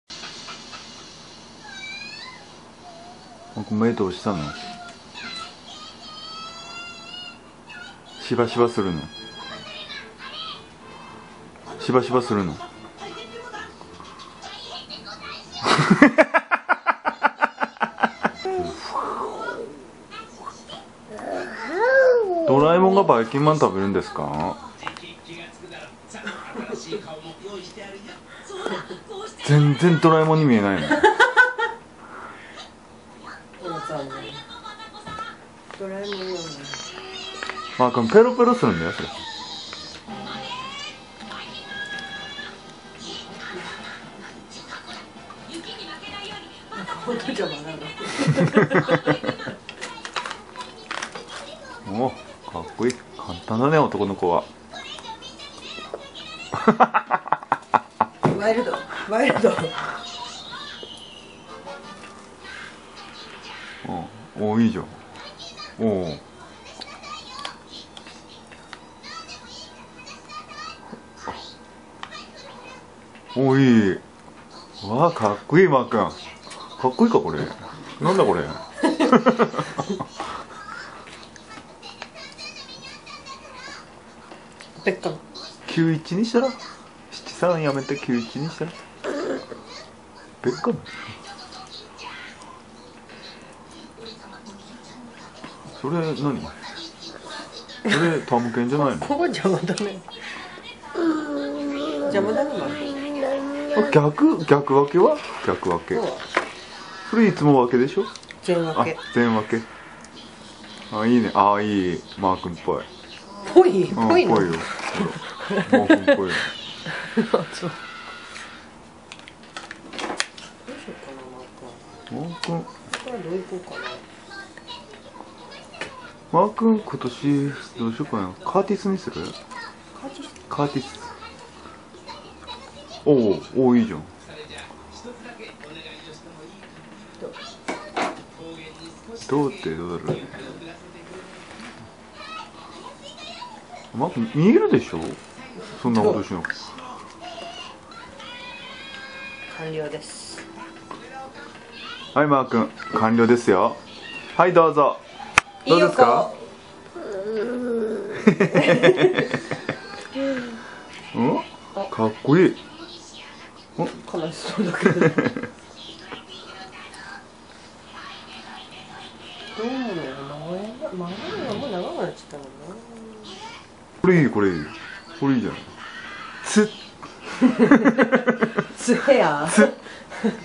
ヘアスタイルYuuma
2014年3月29日/Y:2歳5ヶ月　The mom sets his hairstyle. Hahaha

「つ」・笑